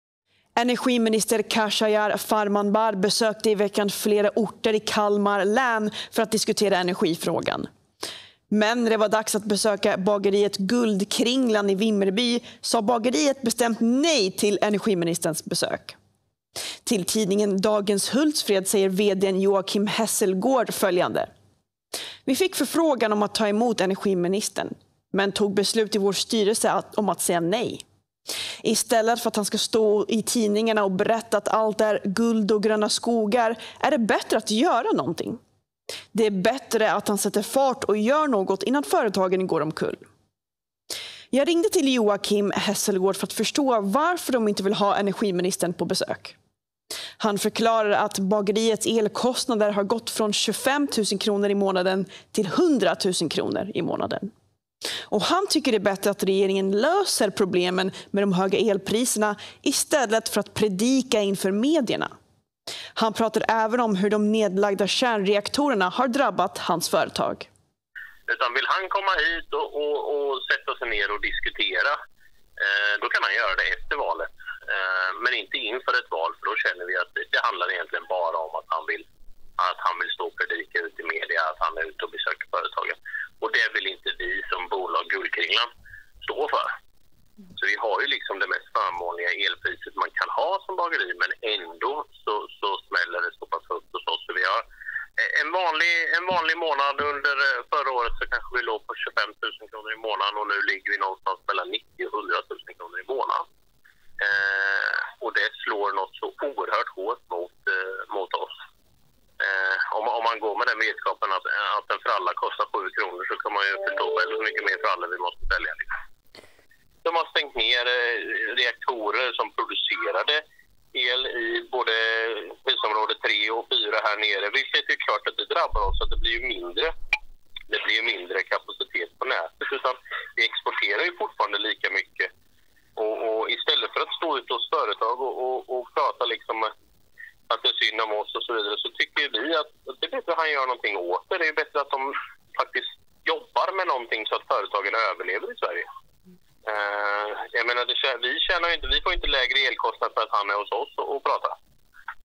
Energiministern Farmanbar portad av bagare i Kalmar
"Istället för att han ska stå i tidningarna och berätta om att allt är guld och gröna skogar är det bättre att göra någonting innan företagen går omkull" säger VD:n Joakim Hesselgård för bageriet Guldkringlan i Vimmerby som tackade nej till energiministern Farmanbars besök.